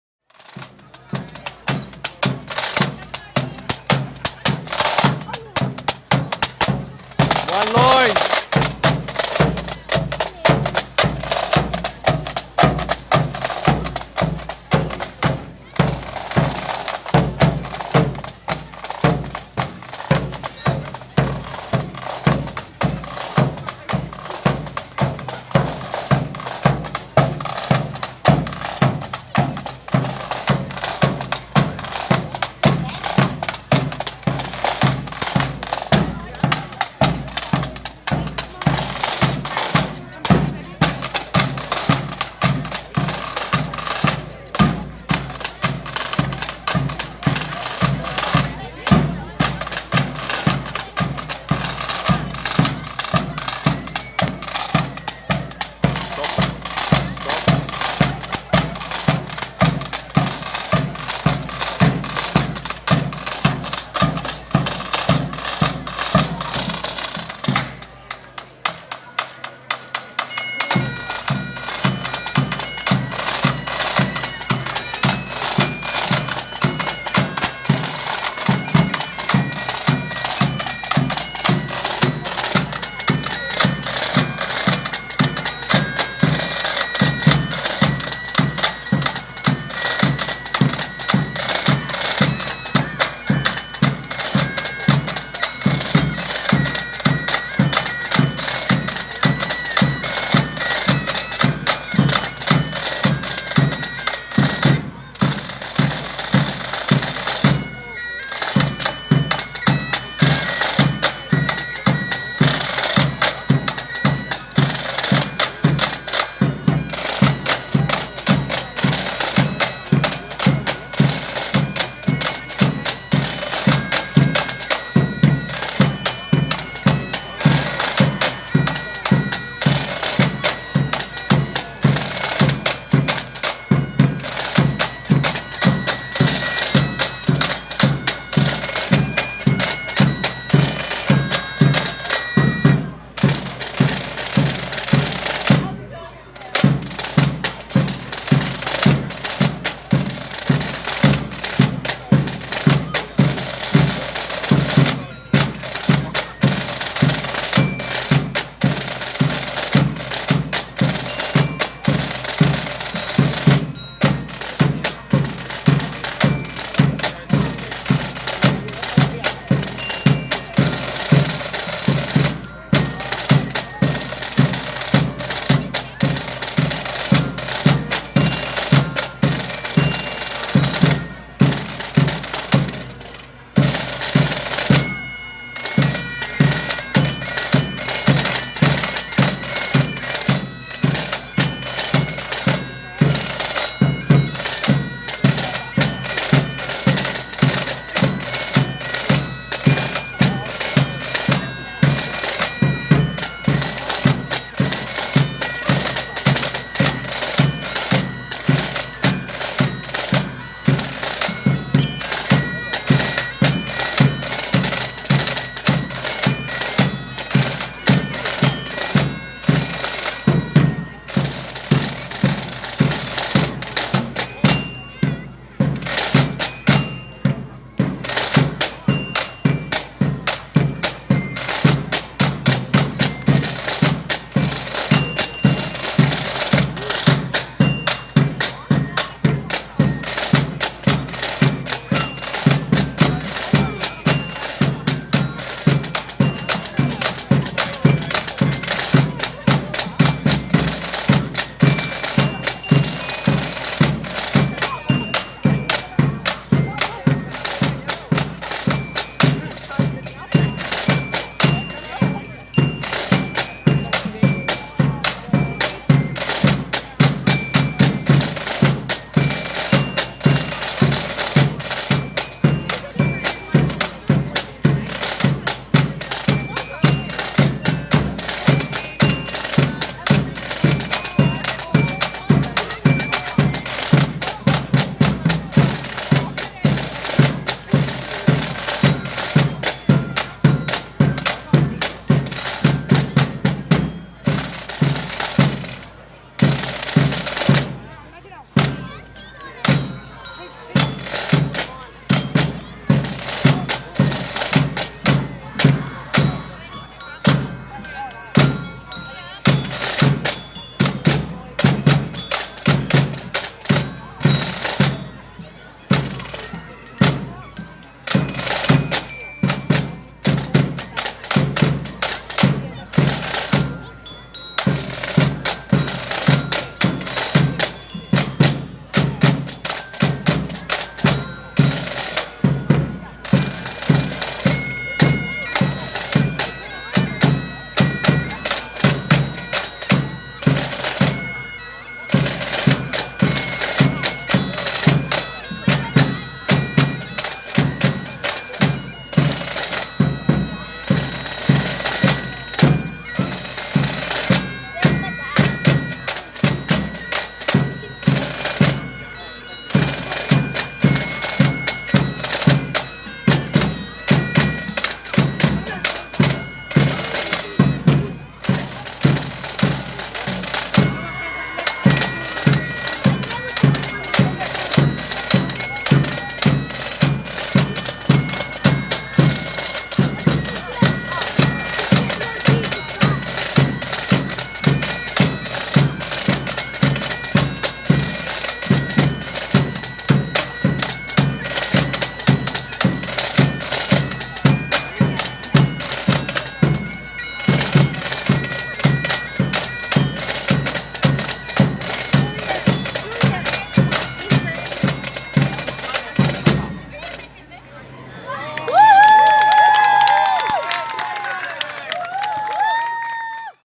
Saint Catherine of Siena School Fair 2019
Have a look at the Squadron taking part in the St Catherine of Siena Prestons Primary School Festival of Fun. The band entertained crowds at the birthday celebration with a variety of tunes, check it out!